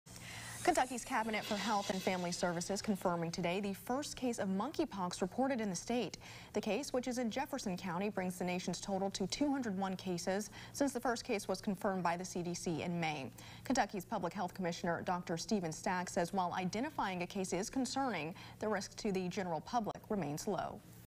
Kentucky reports first probable monkeypox case
The Centers for Disease Control and Prevention (CDC) confirmed the first case of rare monkeypox in May in Massachusetts. On Friday, the Kentucky Cabinet for Health and Family Services confirmed the probable case in Jefferson County.